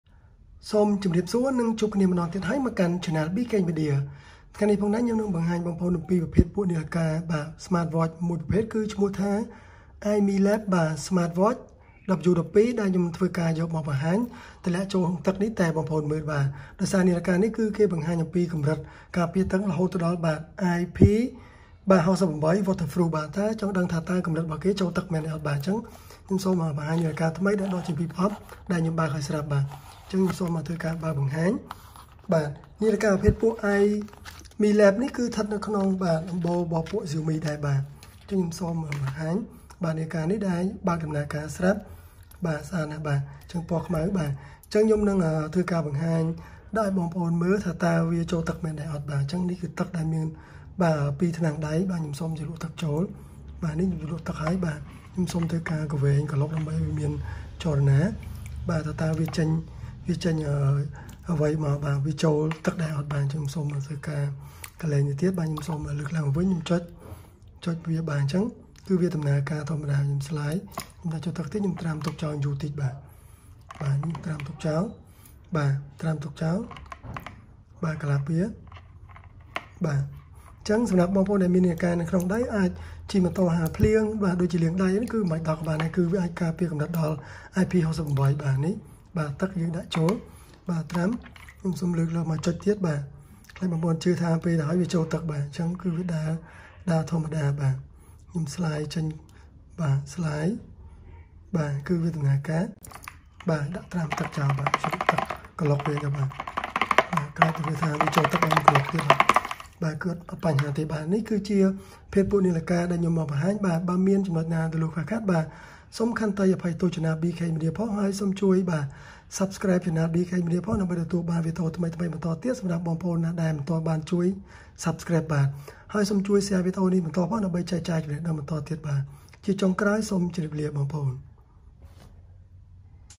សាកល្បងជាមួយទឹកនាឡិកាដៃ IMILAB Smart Watch W12 - Xiaomi IMILAB Smart Watch W12 Water test with IP68
ទស្សនាវីដេអូនេះបងប្អូននិងយល់ដឹងពីៈ សាកល្បងជាមួយទឹកនាឡិកាដៃ IMILAB Smart Watch W12 - Xiaomi IMILAB Smart Watch W12 Water test with IP68. This movie will show you about try to Xiaomi IMILAB Smart Watch W12 Water test with IP68.